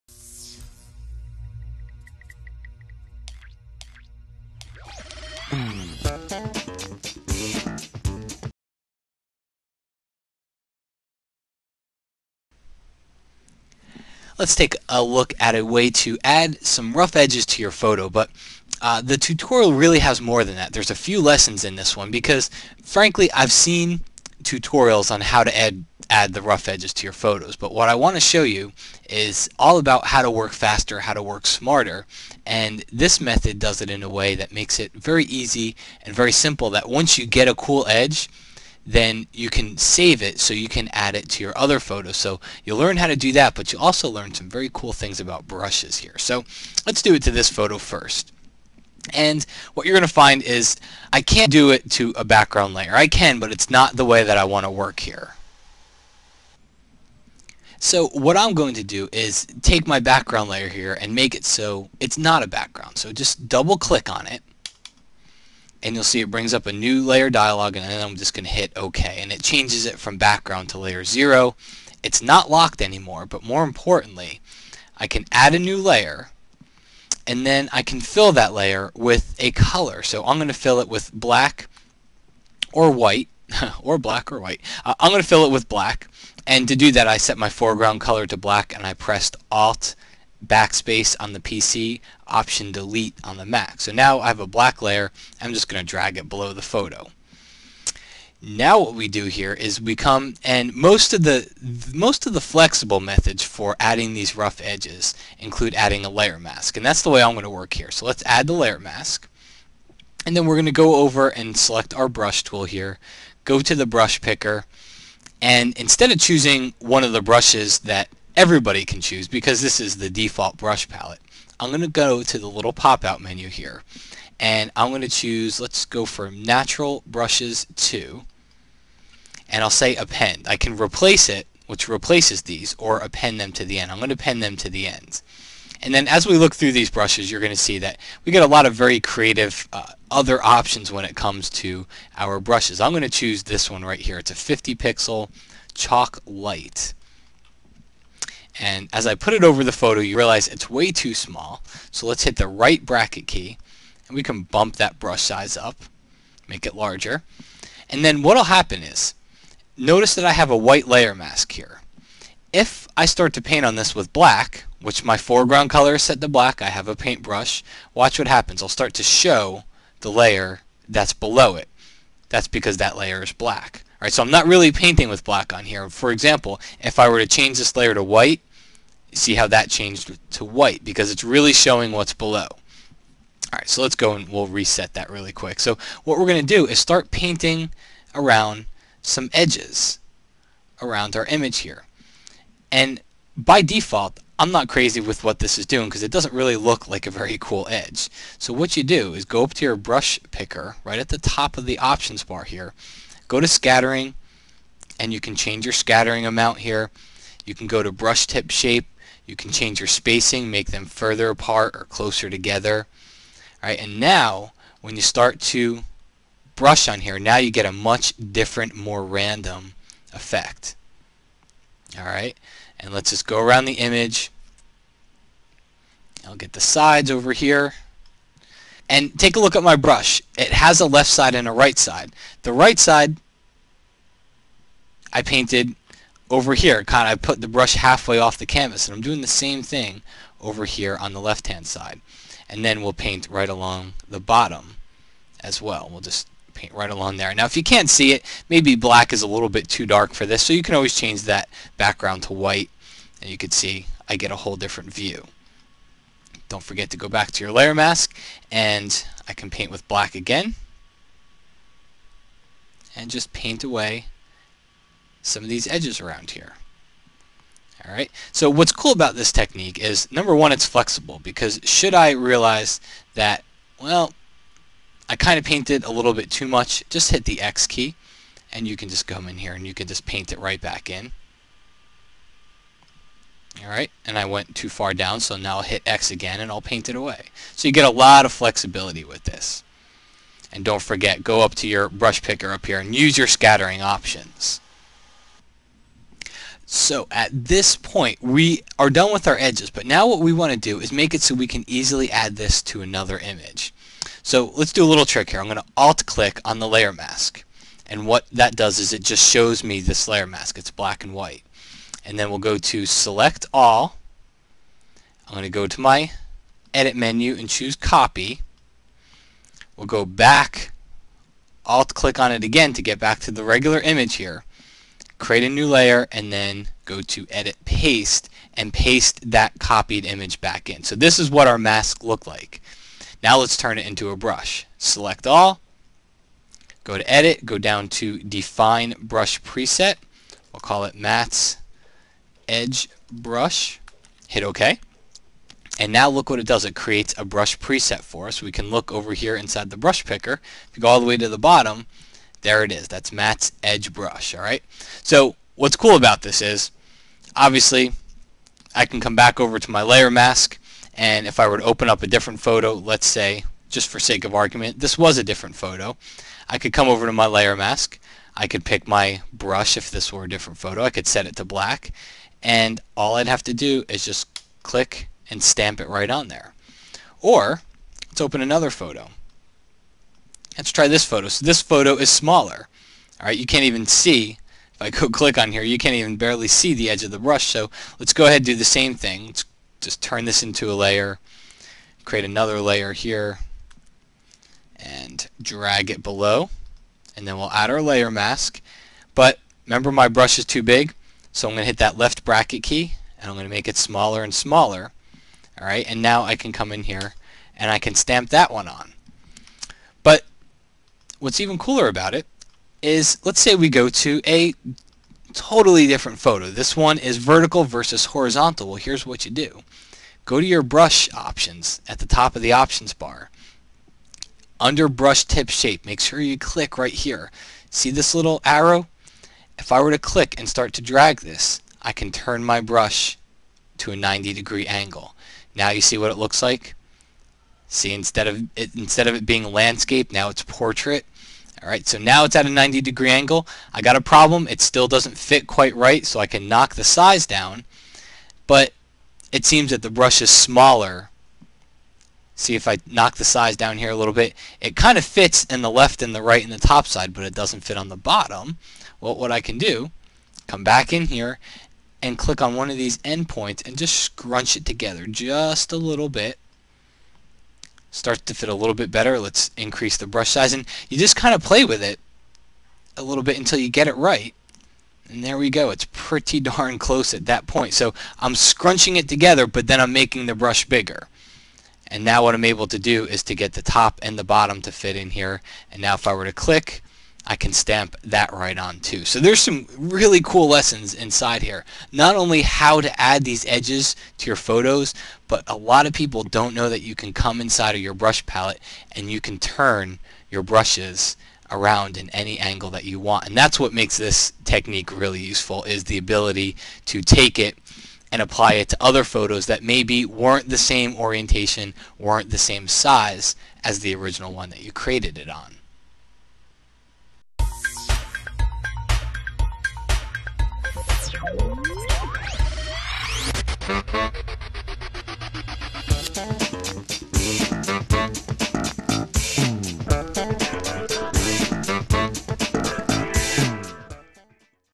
How to Add a Brushed Border
Matt Kloskowski, the education and curriculum developer for the National Association of Photoshop Professionals, takes you step-by-step through this simple demonstration. Showing you how to add rough edges to your photo with a brushed border, and work faster and smarter by creating a border that can be used over and over.